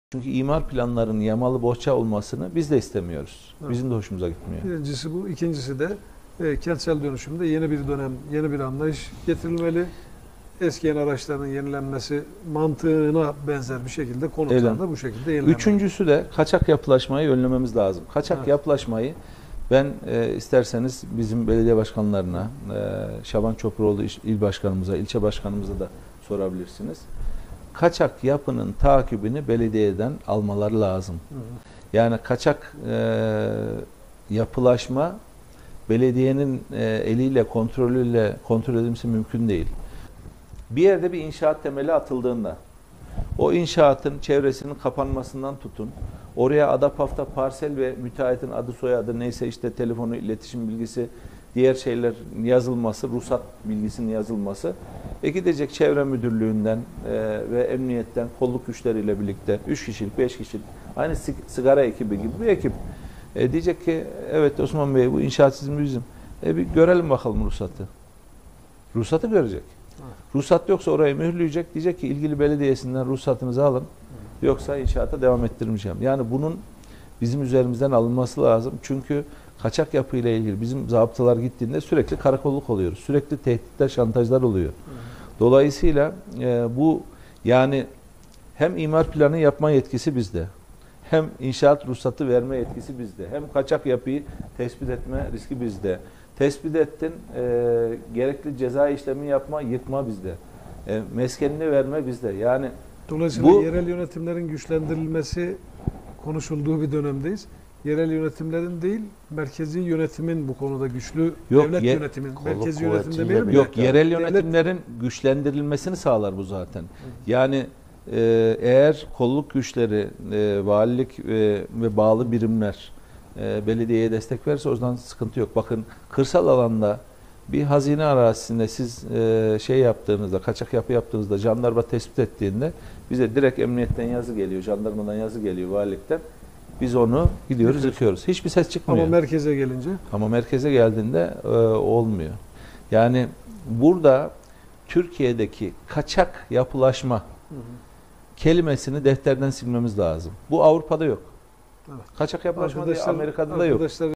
Tehdit ve şantaj var” diyen Palancıoğlu: Bunu belediyeden alın…
Melikgazi Belediye Başkanı Mustafa Palancıoğlu, kaçak yapılaşmayla ilgili dikkat çeken açıklamalar yaptı. Kaçak yapılaşma ile ilgili sürekli tehdit ve şantajların olduğunu ve denetimlerinin belediyelerden alınması gerektiğini söyleyen Başkan Palancıoğlu, ,”Kaçak yapının takibini belediyeden almaları lazım. Bunun belediye eliyle yapılması mümkün değil” dedi.
Palancıoğlu, şunları söyledi: Kaçak yapının takibi belediyelerden alınmalı. Bizim zabıtalarımız gittiğinde sürekli karakolluk oluyoruz. Kaçak yapılaşma ile ilgili sürekli tehdit ve şantajlar oluyor. Hem imar planı yapma yetkisi, hem inşaat ruhsatı verme yetkisi hem kaçak yapıyı tespit etme, gerekli ceza işlemi ile yıkma yetkisi bizde. Ülkede kaçak yapılaşma kelimelerini defterden silmemiz gerek. Bu kelimeler Avrupa da ve Amerika da yok” diye konuştu.